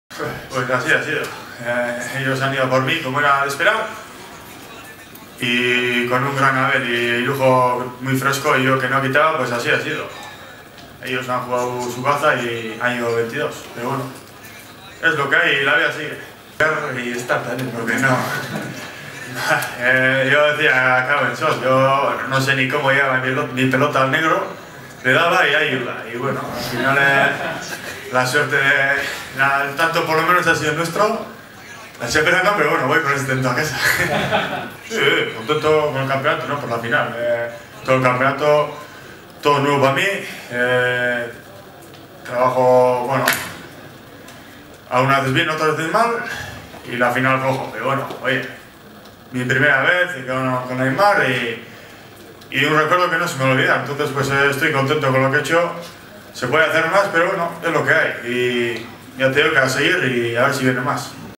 Aretxabaleta: "Ellos han ido a por mi y yo no he quitado"
Declaraciones de Aretxabaleta tras caer derrotado por 22-13 junto con Olaizola II ante Irujo y Barriola en la Final del Parejas 2014